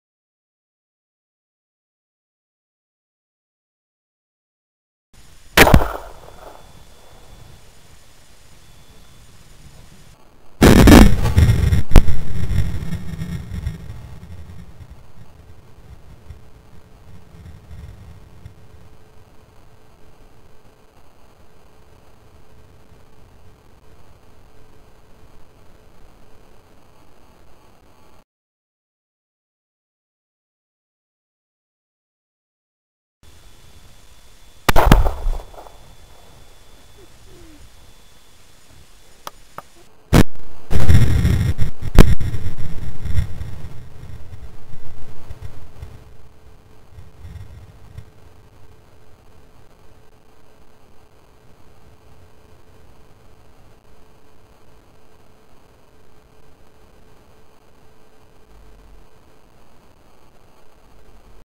Shooting Paint Cans!
Shooting Some paint cans with a Ruger Mini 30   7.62x39